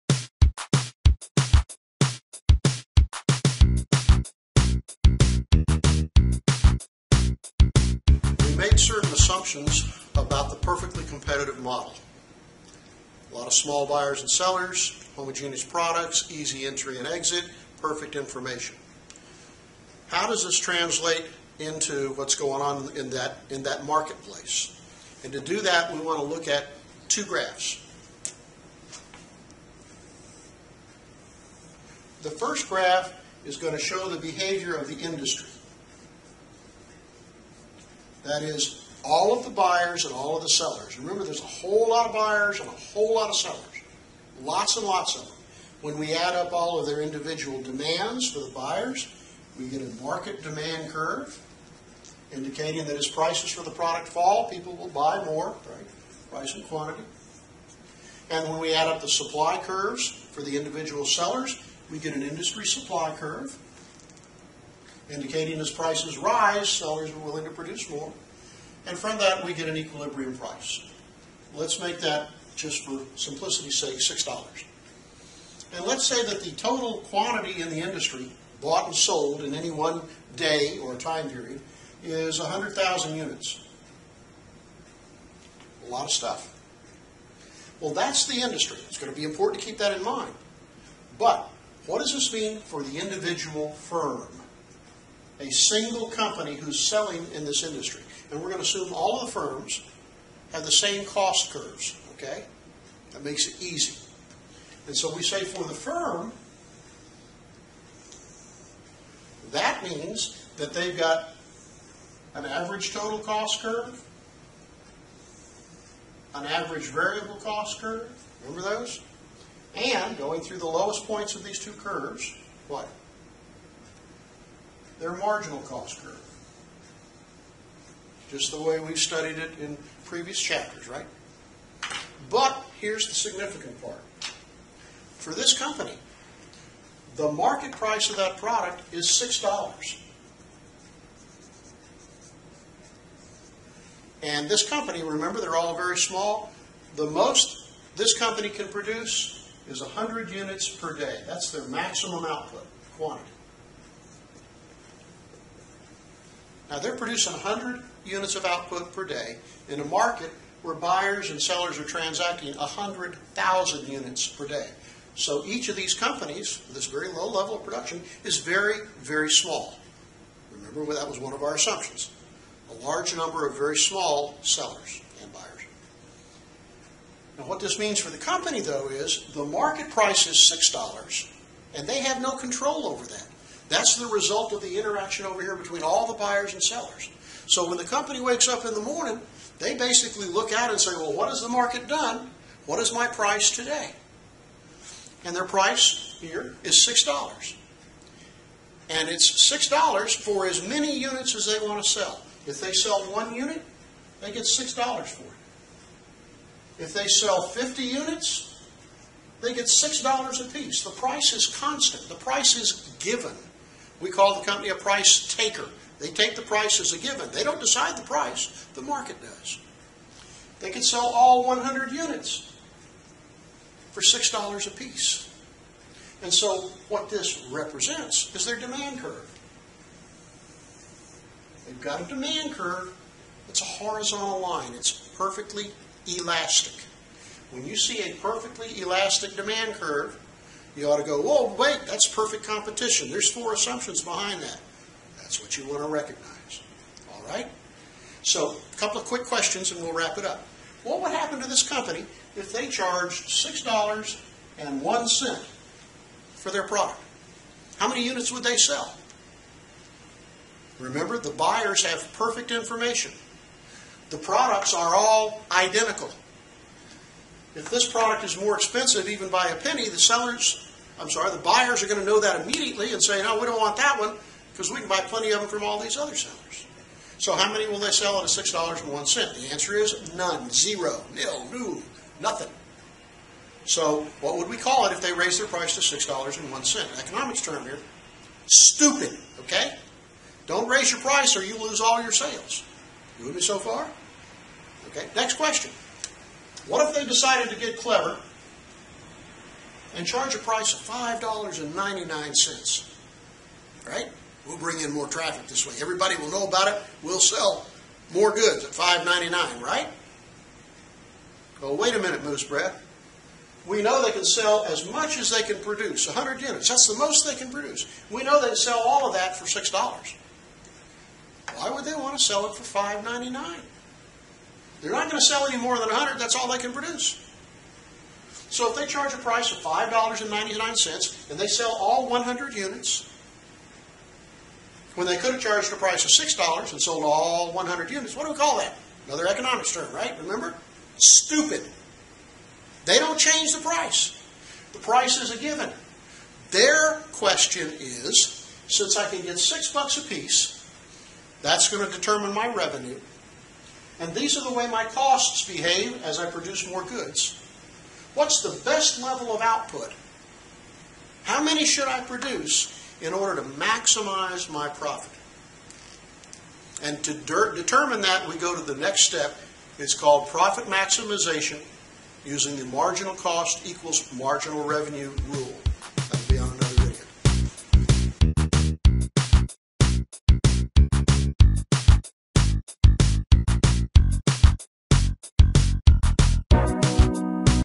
Perfect Competition (2) - The Firm and the Industry
How the perfectly competitive firm is faced with taking whatever price the market sets.